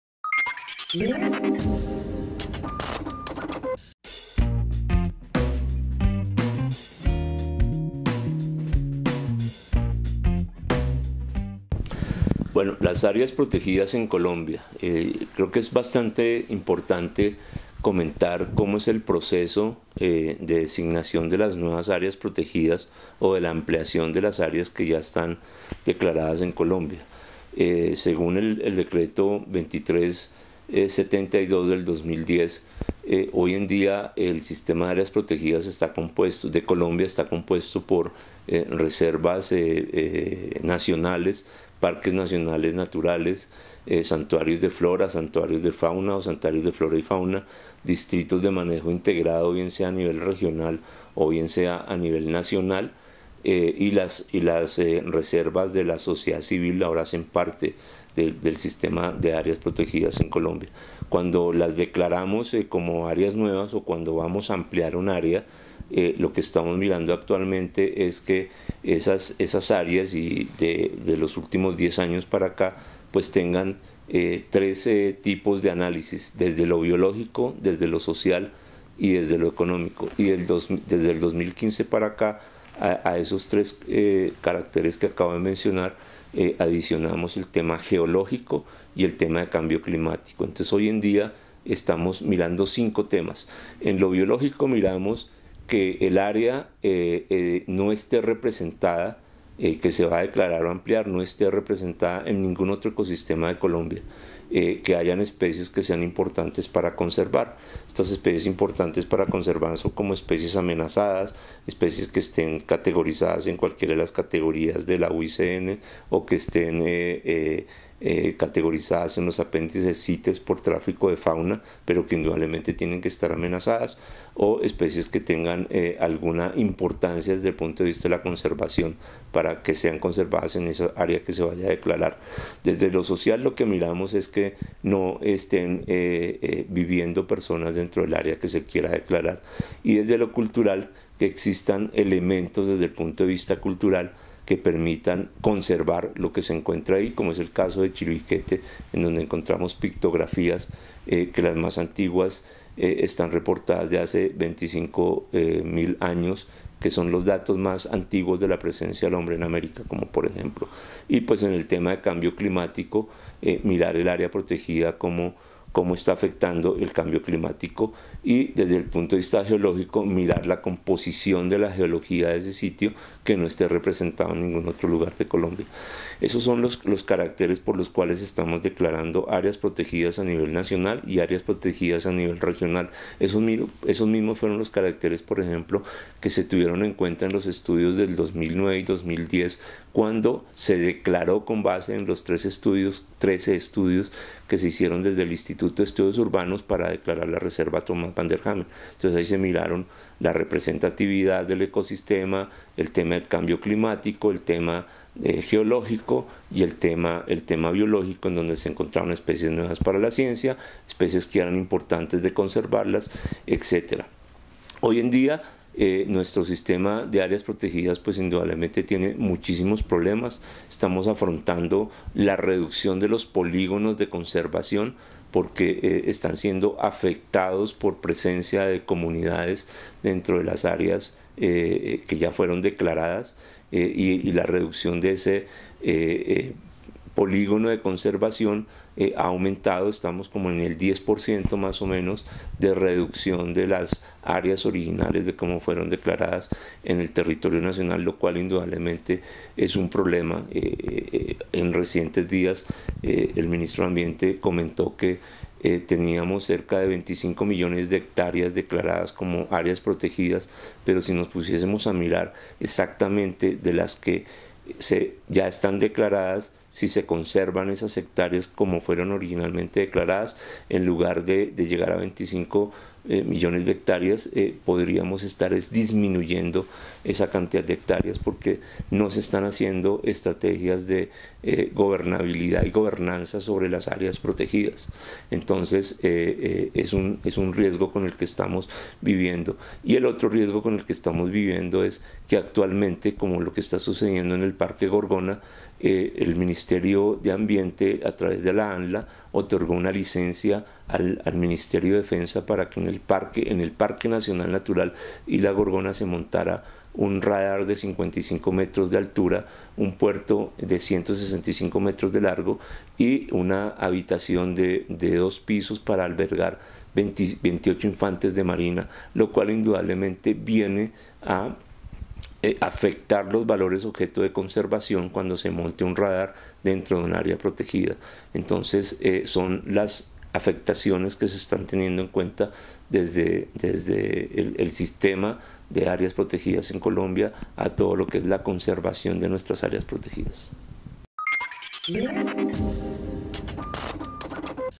100ExpertosDicen Gobernabilidad y gobernanza en las áreas protegidas - Gonzalo Andrade
Profesor del Instituto de Ciencias Naturales de la Universidad Nacional de Colombia. Magíster en Ciencias - Biología.